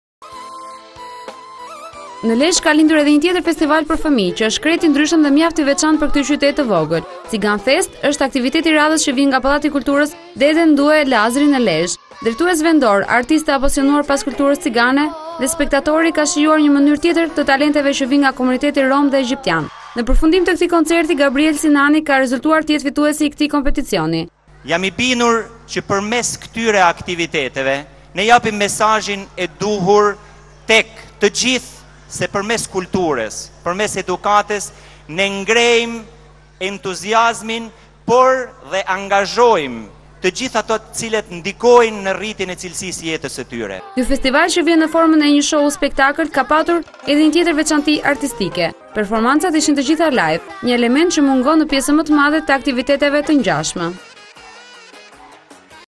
A1 Report - Lezhë, 'Cigan Fest' evidenton artistët e komunitetit rom e egjiptian
Në Lezhë ka lindur edhe një tjetër festival për fëmijë, që është krejt i ndryshëm dhe mjaft i veçantë për këtë qytet të vogël. 'Cigan Fest' është aktiviteti i radhës që vjen nga pallati i kulturës "Dede Ndue Lazri" në Lezhë.

Drejtues vendor, artistë të apasionuar pas kulturës cigane dhe spektatori ka shijuar një mënyrë tjetër të talenteve që vinë nga komuniteti rom dhe egjiptian.

Në përfundim të këtij koncerti, Gabriel Sinani, ka rezultuar të jetë fituesi  i këtij kompeticioni.

Jam i bindur se përmes këtyre aktiviteteve ne japim mesazhin e duhur tek të gjithë se përmes kulturës, përmes edukatës ngremë entuziazmin, por angazhojmë të gjitha ata që ndikojnë në rritjen e cilësisë së tyre.

Ky festival që vjen në formën e një spektakli ka patur edhe një tjetër veçanti artistike, performancat ishin të gjitha live, një element që mungon në pjesën më të madhe të aktiviteteve  të ngjashme.